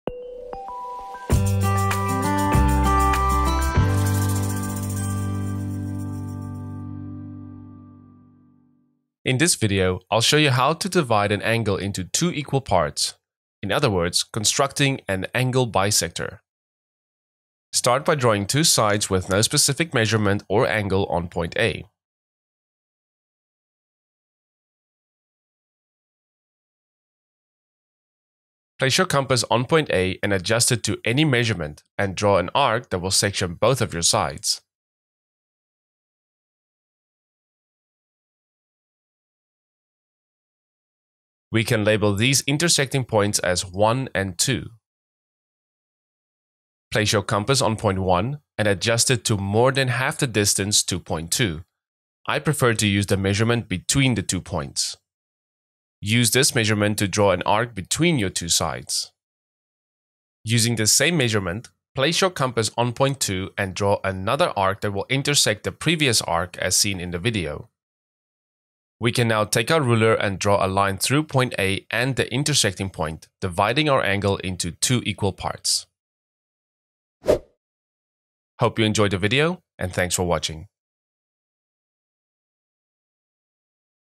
How to divide an angle into two equal parts - Angle Bisector
In this video, I'll show you how to construct an angle bisector or how to divide an angle into two equal parts. This angle bisector is also useful during other constructions and drawing methods. Dividing an angle in two equal part can easily be done using your compass. Hope you enjoyed the video and find it helpful.